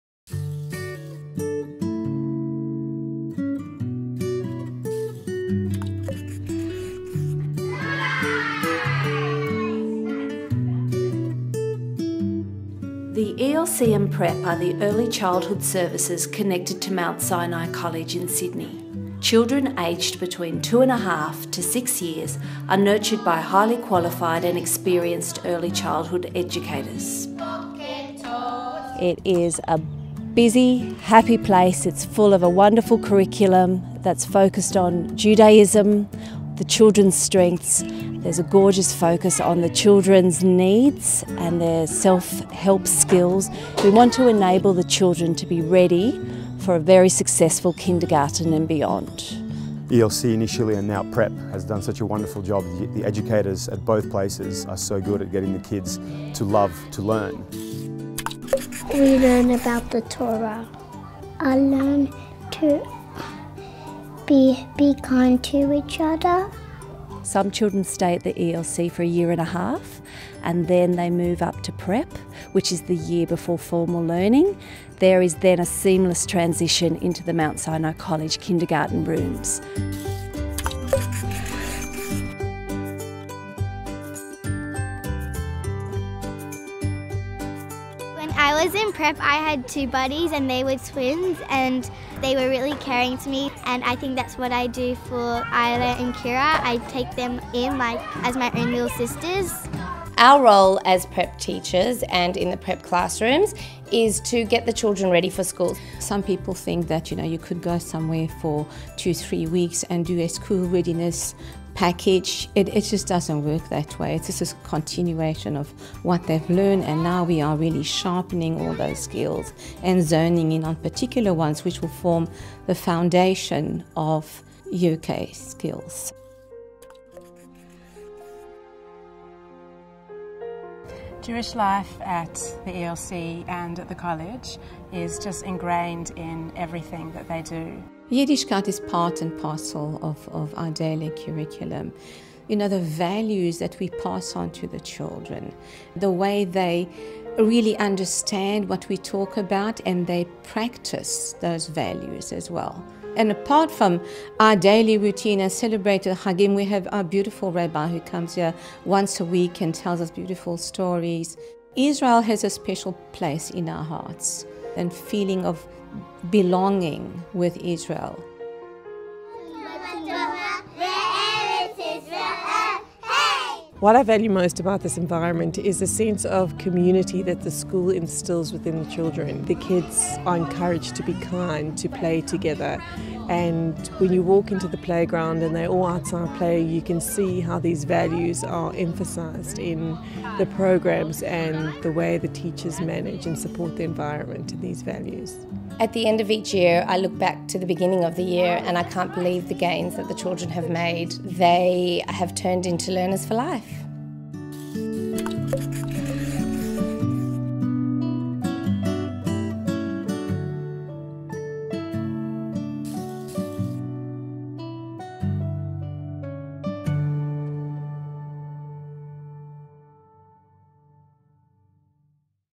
MT SINAI ELC and PREPARATORY
Mount Sinai College's Early Learning Centre and Preparatory Classes are now enrolling students from two and a half years of age.

Our Philosophy that children have the right to learn, feel loved, respected and to have a voice in order to feel safe and secure underpins all that we do at the College. Each child is recognised as unique. Children are supported so they develop autonomy, resilience and the ability to make choices and decisions.